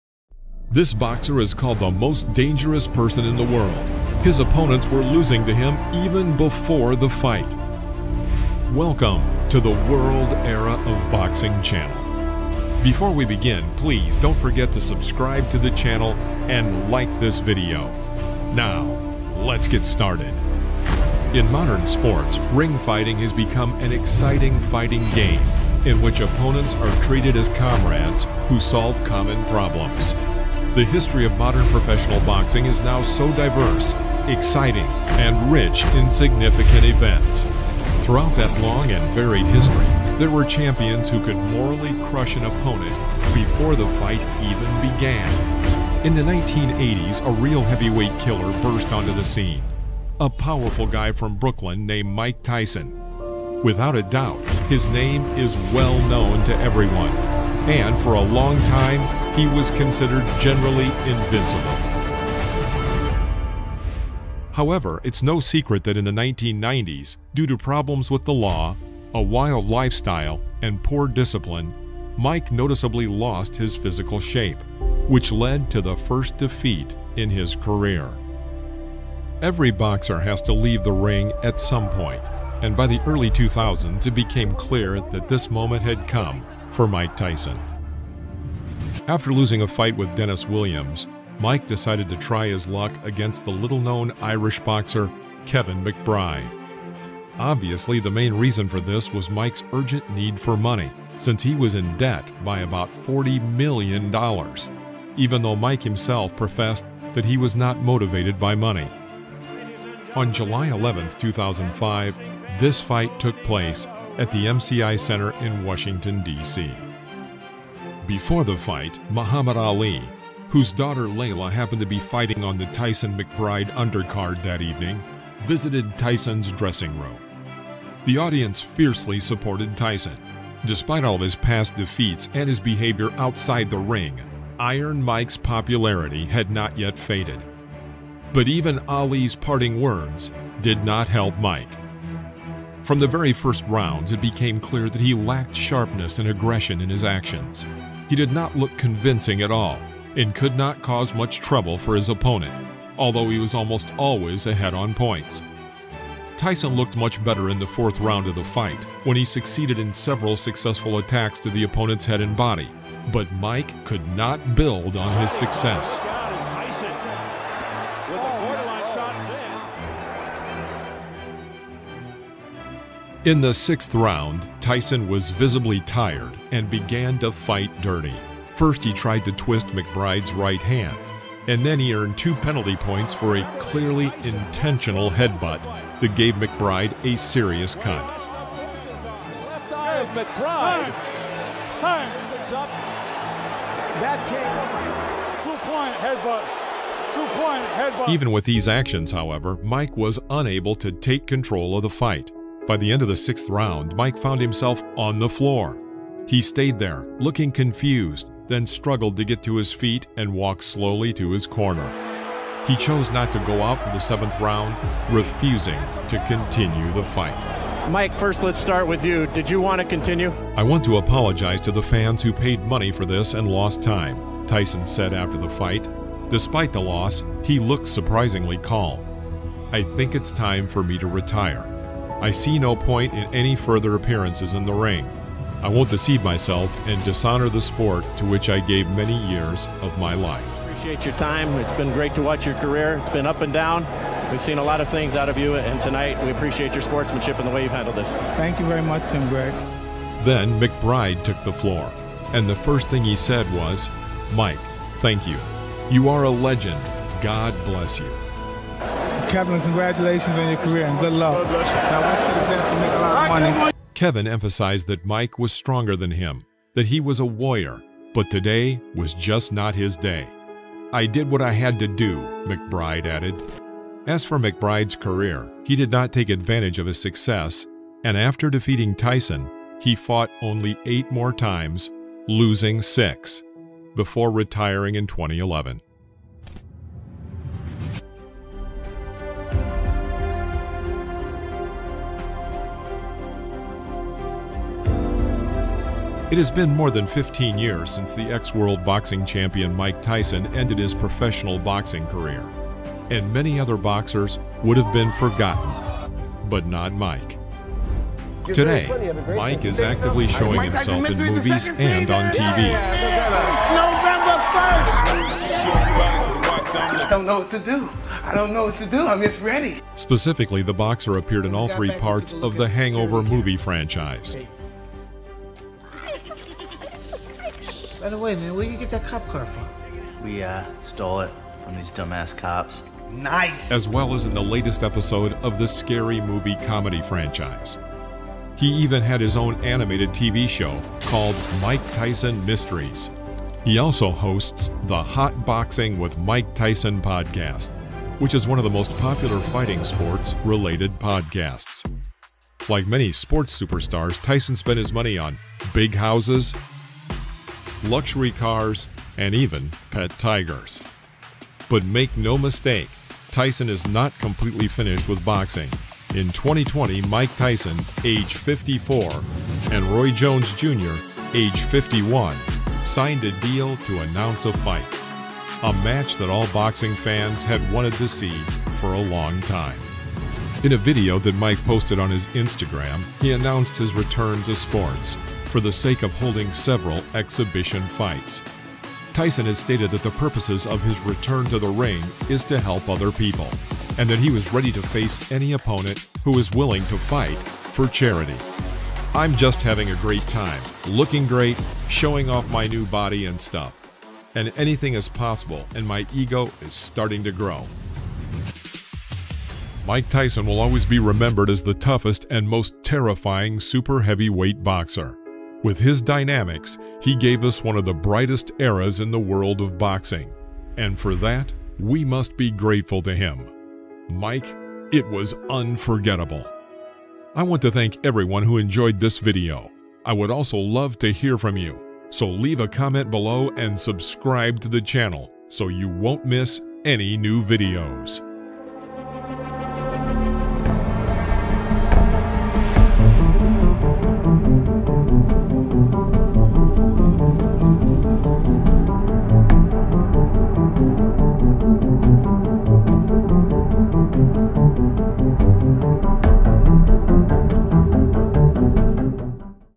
Mike Tyson’s Last Fight – A Farewell to a Legend!
Yeah, that fight was a tough one for Tyson fans. He started strong, but by the later rounds, it was clear that he wasn’t the same ferocious fighter from his prime. Kevin McBride used his size and durability to wear Tyson down, and when Tyson didn’t come out for the 7th round, it was the end of

That fight was also Tyson’s last professional bout, marking the conclusion of a legendary but turbulent career. It wasn’t the farewell fight fans had hoped for, but it cemented his place in boxing history as one of the sport’